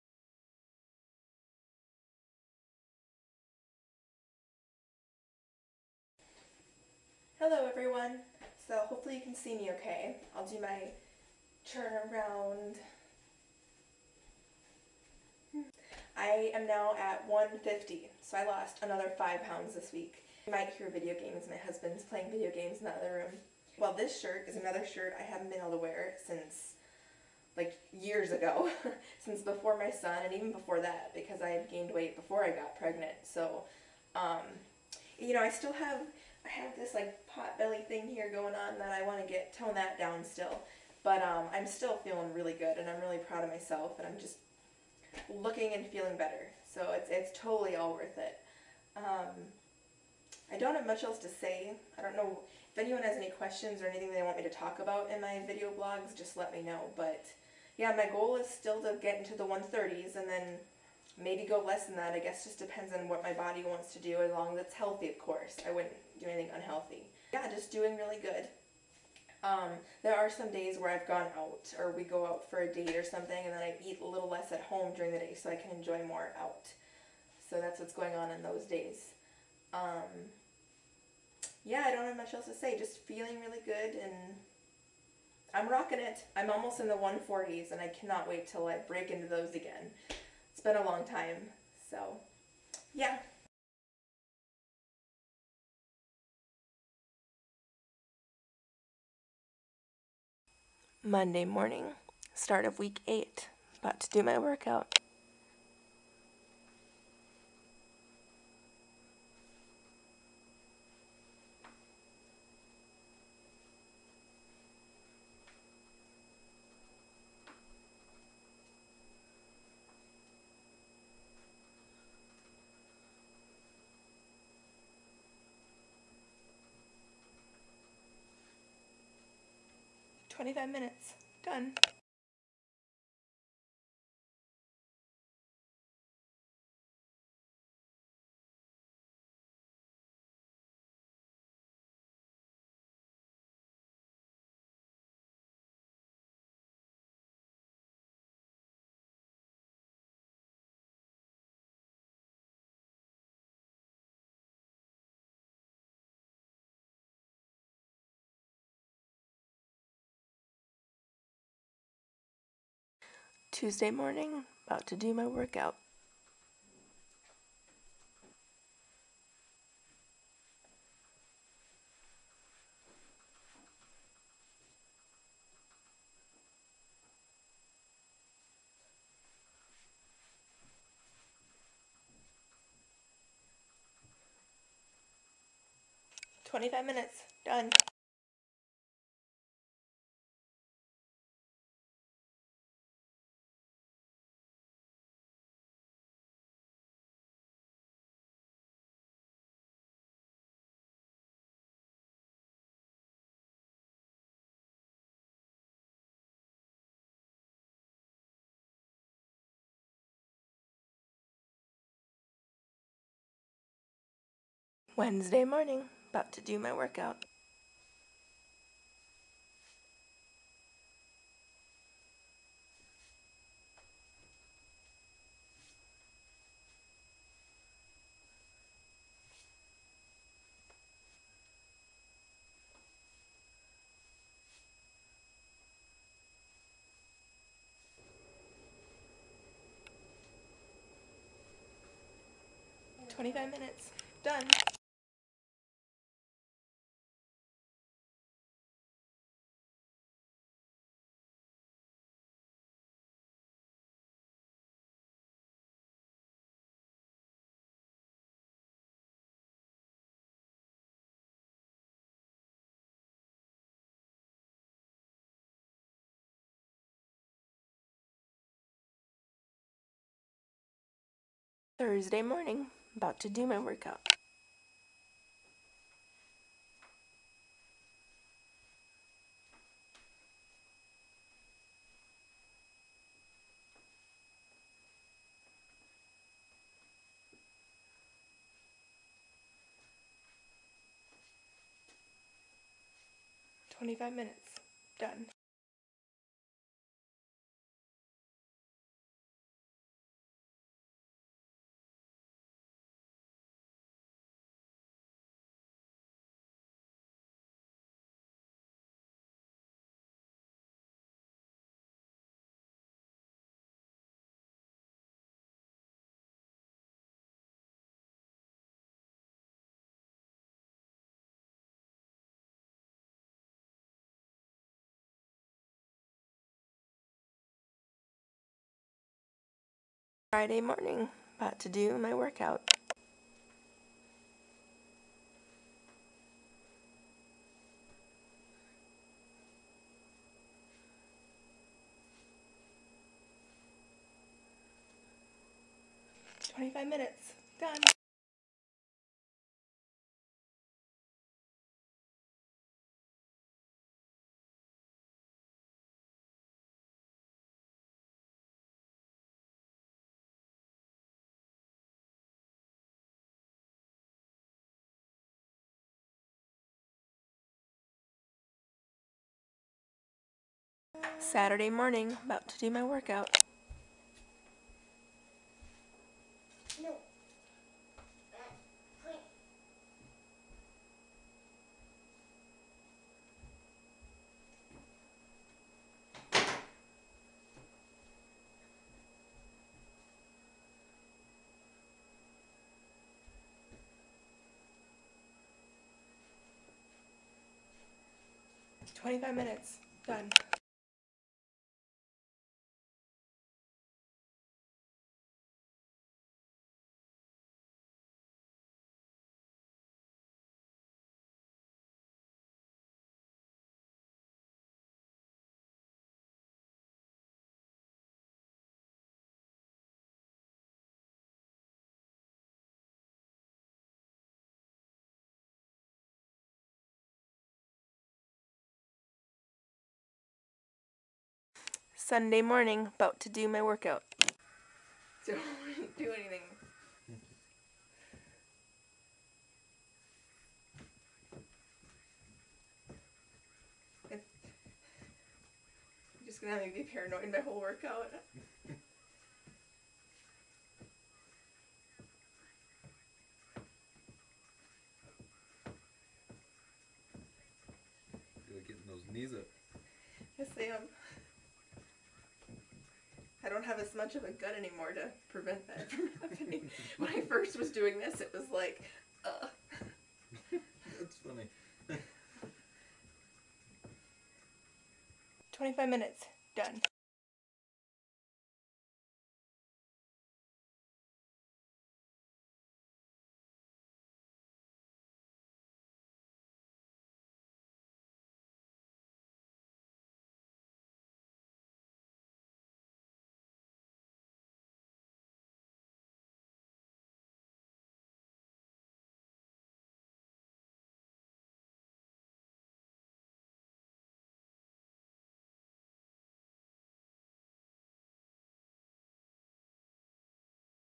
My Weight Loss Journey ~ Week 8 Check In (35 lbs lost so far!)
150!! I lost another 5 lbs this week, bringing my total up to 35 lbs lost so far! Cannot wait to break into the 140s. Enjoying every moment of this weight loss journey :) Going strong. I so got this.